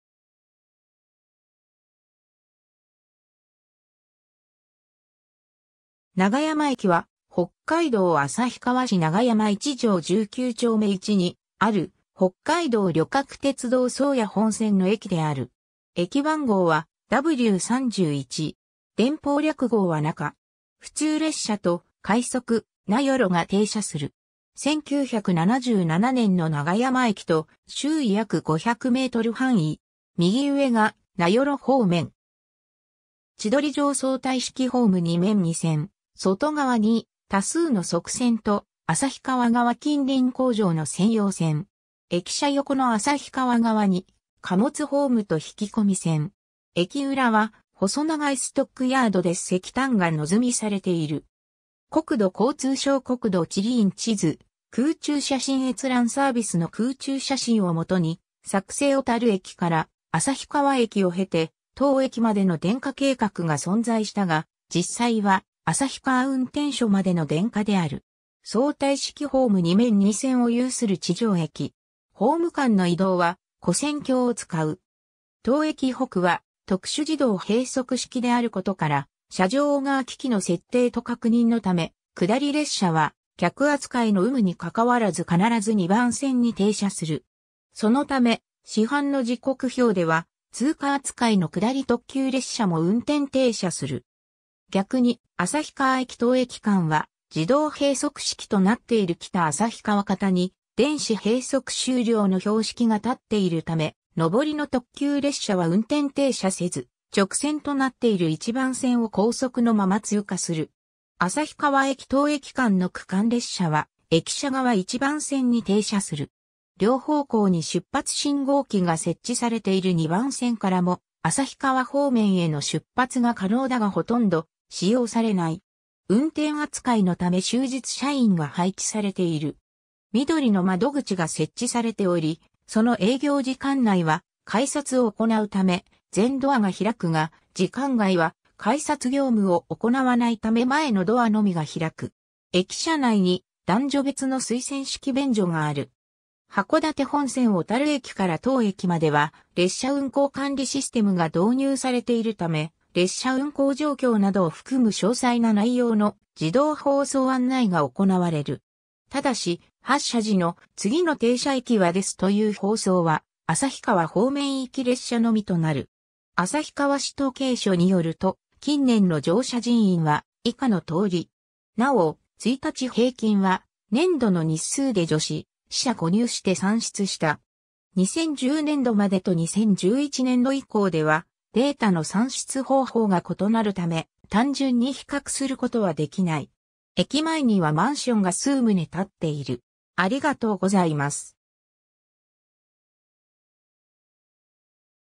永山駅
永山駅（ながやまえき）は北海道旭川市永山1条19丁目1にある北海道旅客鉄道（JR北海道）宗谷本線の駅である。
駅番号はW31。
電報略号はナカ。
普通列車と快速「なよろ」が停車する。
 1977年の永山駅と周囲約500m範囲。
右上が名寄方面。
千鳥状相対式ホーム2面2線、外側に多数の側線と旭川側近隣工場の専用線。
駅舎横の旭川側に貨物ホームと引込み線。
駅裏は細長いストックヤードで石炭が野積みされている。
国土交通省 国土地理院 地図・空中写真閲覧サービスの空中写真を基に作成 小樽駅から旭川駅を経て当駅までの電化計画が存在したが、実際は旭川運転所（北旭川駅付近）までの電化である。
ホーム間の移動は跨線橋を使う。
 当駅以北は特殊自動閉塞式（電子閉塞）であることから、車上側機器の設定と確認のため、下り列車は客扱いの有無にかかわらず必ず2番線に停車する（2番線の北永山方に「電子閉そく開始」の標識が立っている）。
そのため、市販の時刻表では通過扱いの下り特急列車も運転停車する。
 逆に、旭川駅 - 当駅間は自動閉塞式（PRC付帯）となっている（1番線の（貨）北旭川方に「電子閉そく終了」の標識が立っている）ため、上りの特急列車は運転停車せず、直線となっている1番線を高速のまま通過する。
旭川駅 - 当駅間の区間列車は駅舎側1番線に停車する。
両方向に出発信号機が設置されている2番線からも旭川方面への出発が可能だがほとんど使用されない（過去には当駅止まりの連結の切り離しの際に使用されたケースがある）。
 運転扱いのため終日社員が配置されている。
みどりの窓口（営業時間:7時00分～18時50分）が設置されており、その営業時間内は改札を行うため全ドアが開くが、時間外は改札業務を行わないため前のドアのみが開く。
駅舎内に男女別の水洗式便所がある。
 函館本線小樽駅から当駅までは列車運行管理システムが導入されているため、列車運行状況などを含む詳細な内容の自動放送案内が行われる。
ただし、発車時の「次の停車駅は○○です」という放送は旭川方面行き列車のみとなる。
 「旭川市統計書」によると、近年の乗車人員は以下の通り。
なお、1日平均は年度の日数で除し、四捨五入して算出した。
2010年度までと2011年度以降ではデータの算出方法が異なるため、単純に比較することはできない。
 駅前にはマンションが数棟建っている。